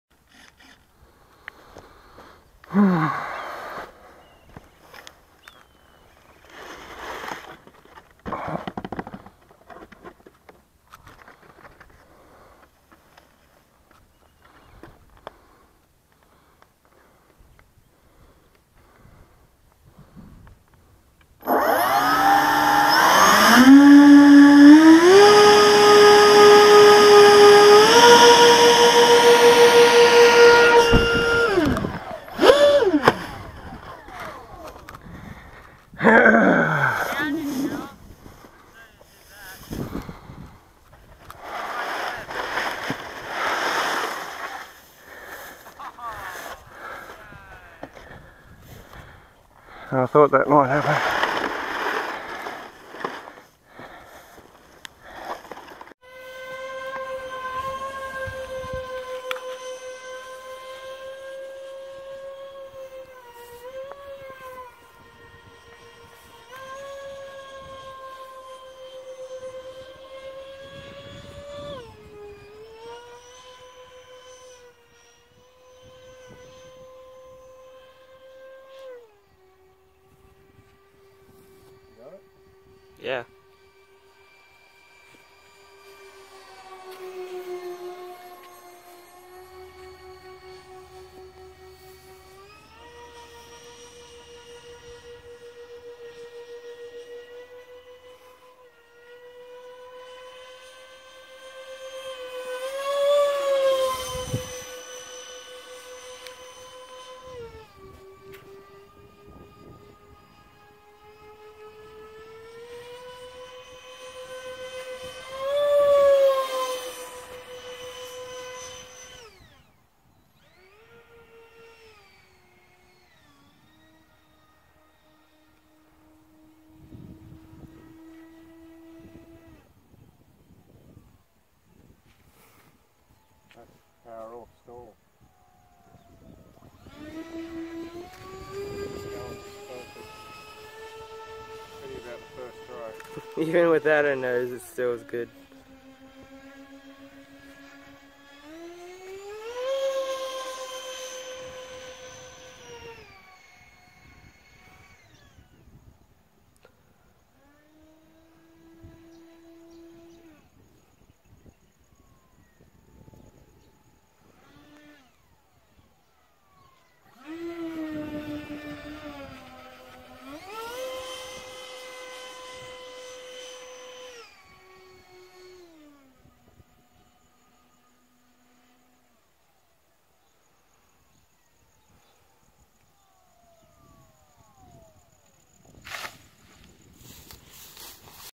Rc plane Me 262GH III Vtail maiden
First throw was not good. but repairable, decided to give it go with a missing nose... flys well